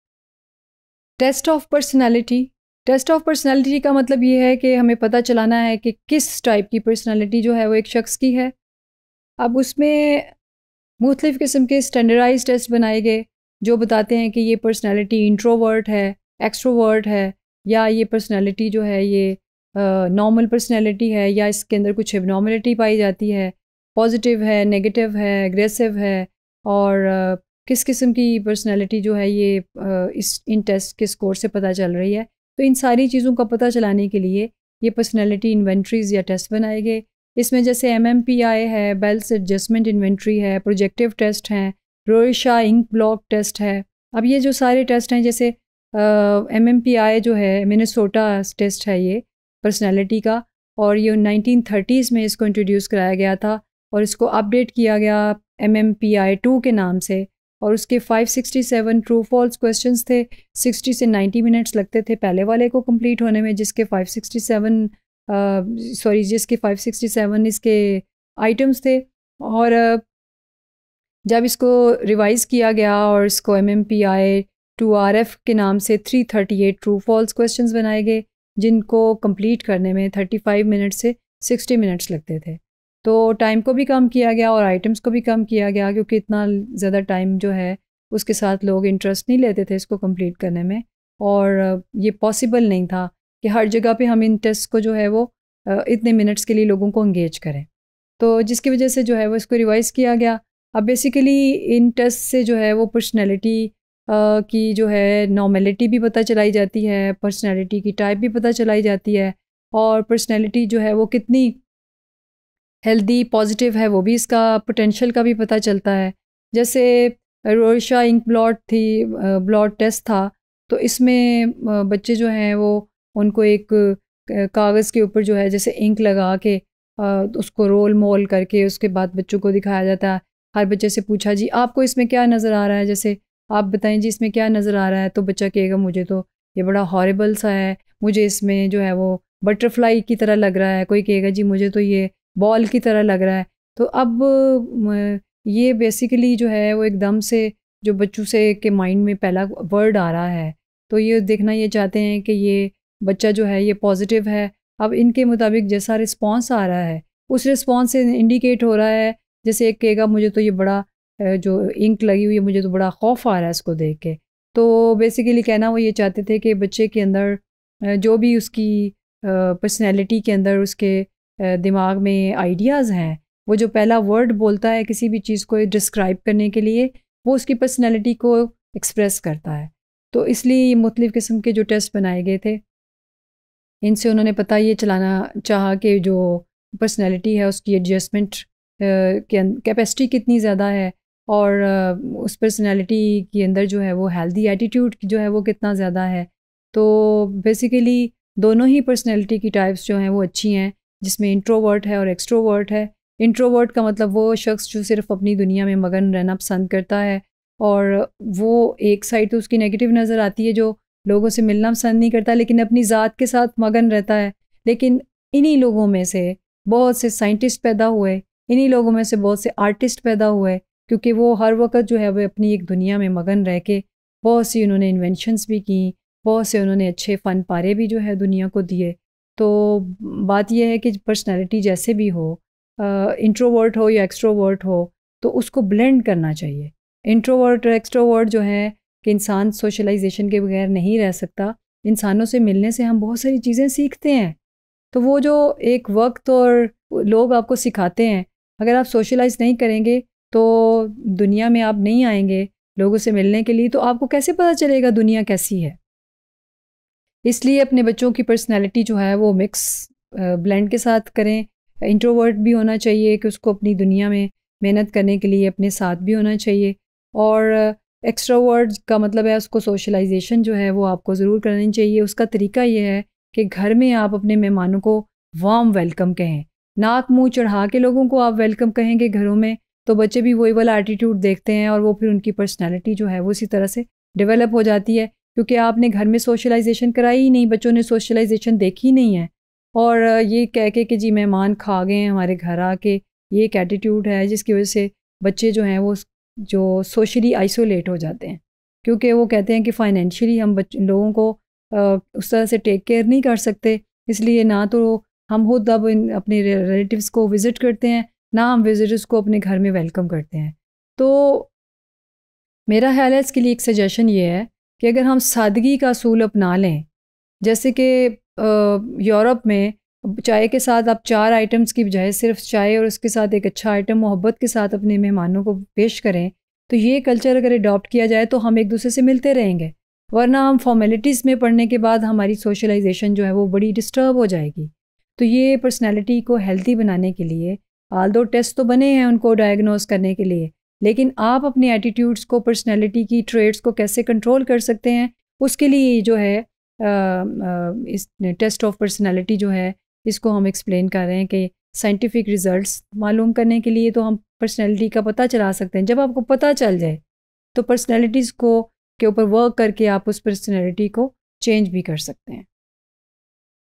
Test of Personality | Human Development and Learning | EDU302_Topic124
Topic124 - Test of Personality
By Dr. Sumaira Rashid